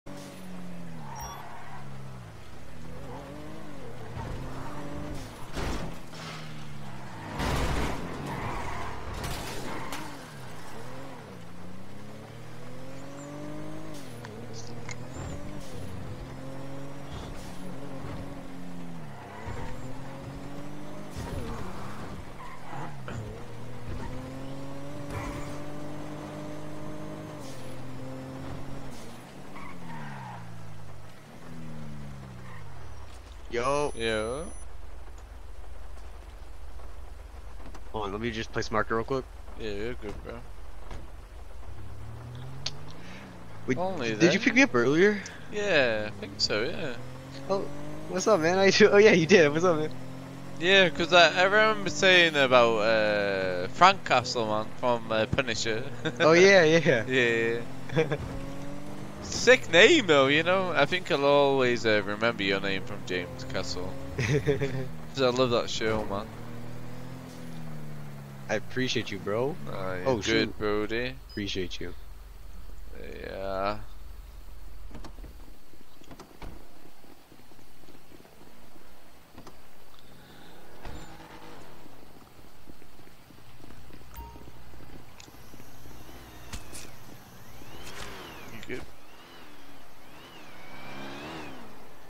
L.A Noire RP RDM TAXIK
he got 300coms then they added all of his shit together then he got a permban